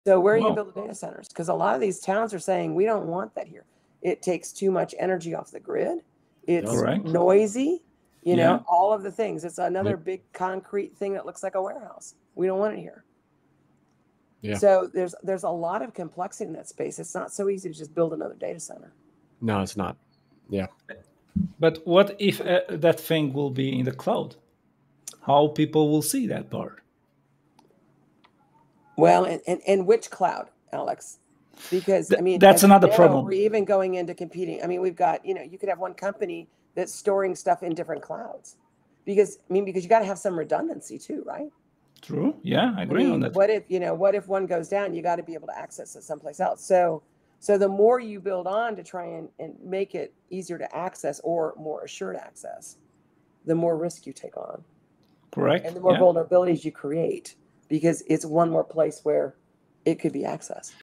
Data Centers: Challenges, Cloud Solutions, and Future Risks
We delve into the complexities of data center construction. Our discussion covers the challenges of building data centers, focusing on energy consumption, community opposition, and cloud solutions, emphasizing the importance of redundancy and risk management within the cloud environment. DataCenters CloudComputing TechChallenges DataStorage CloudSolutions Redundancy ITInfrastructure TechDiscussion DigitalTransformation TechNews Bonnie Stitt, Aaron Stillwell. Alexandru Mavrodineanu